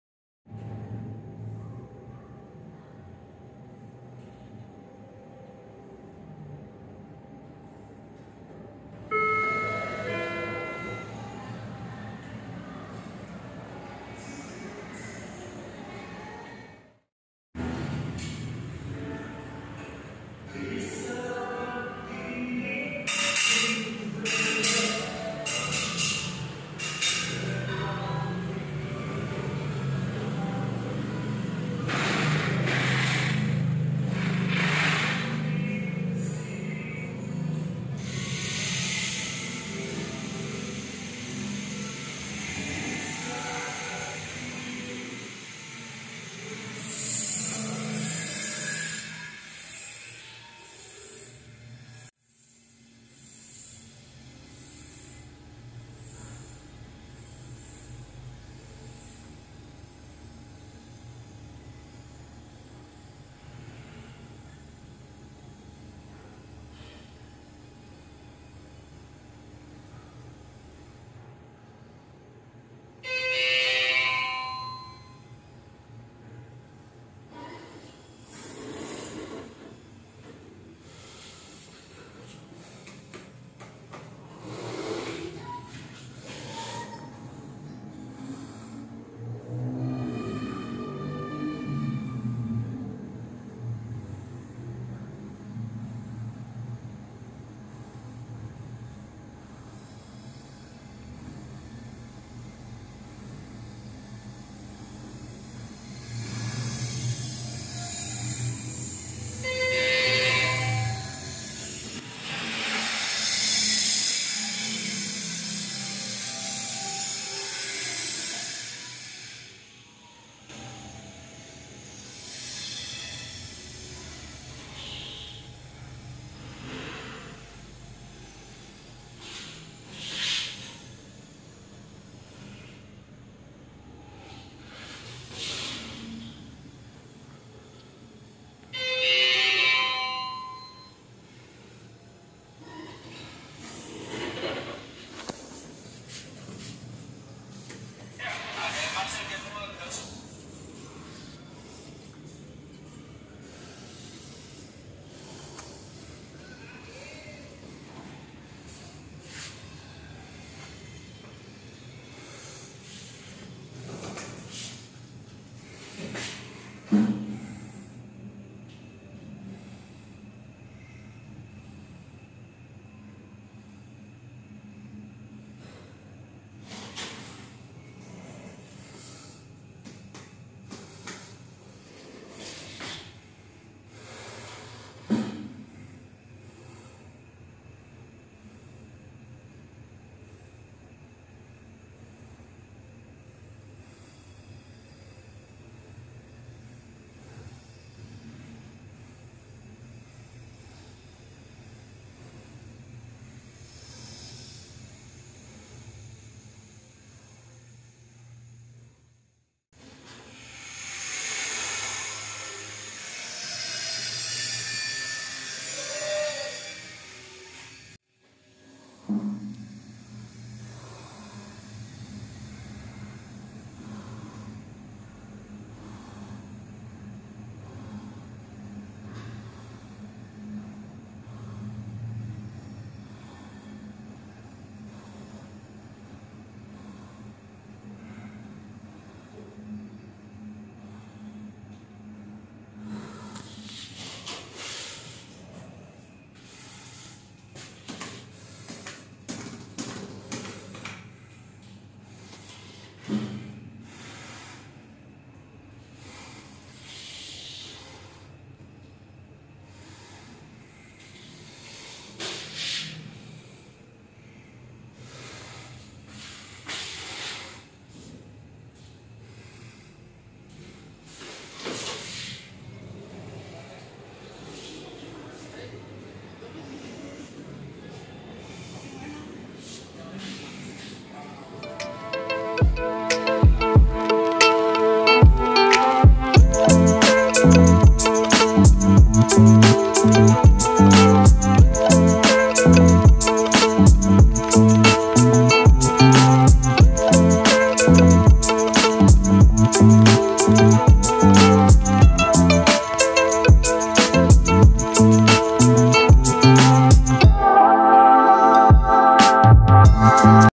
[4K] Hyundai STVF Scenic Lifts - Pasar HWI Lindeteves, Jakarta
Just a 4K remastered of the scenicelevator at one of the hardware market in Glodok area

[Video Information]
• Shooting Dates: April 1st 2023
• Camera: Vivo S1 Pro
• Edited with: Filmora 9

[Building Information]
• Building Name: HWI Lindeteves
• Building Type: Trade Market
• Location: Glodok (Hayam Wuruk Rd.)

[Lift / Elevator Information]
• Brand: Hyundai
• Model / Fixtures: STVF Geared Traction / Hyundai 2nd Gen w/ STEP EB 960
• Installer: PT Superhelindo Jaya Perkasa
• Installation Year: 2013
• Type: Passenger Scenic
• Capacity: 15 Persons / 1000 Kg
• Serving: B, D(GF), L1, L2, L3, L3A (6 Floors)
• Non-Serve Floors: None
• History:
   - 1991: Installed by GoldStar LVO
   - 2010s (?): Modernized by Unknown Generic
   - 2013: Completely replaced by Hyundai STVF

-Like my Facebook Page: VT Elevators (@vtelevators93)

Note: You CANNOT Reuse or Reupload this content without the Admin's permission
Always Remember: Lift/Elevator Filming it's NOT A CRIME!